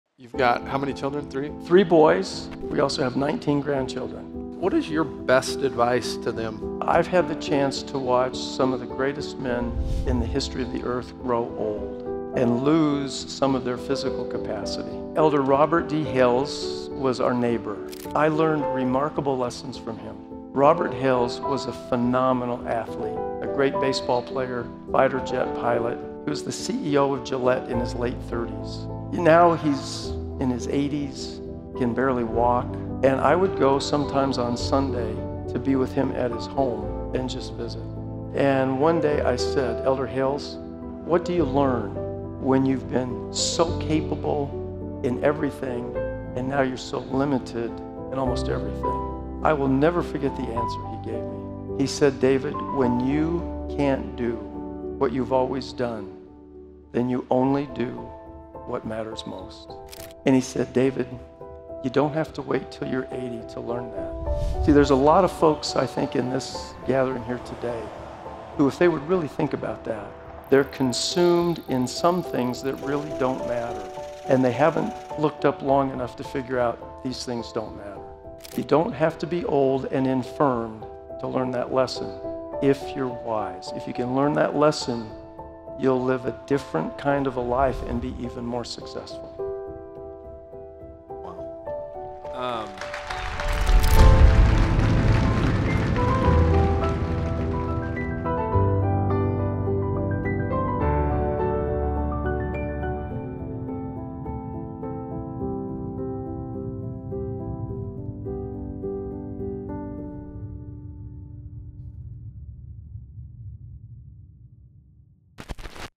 Elder Bednar's Best Advice to His Children // Silicon Slopes Summit w/Ryan Smith
This is taken from an interview between Elder Bednar and Ryan Smith at Silicon Slopes Summit, the full interview can be seen here: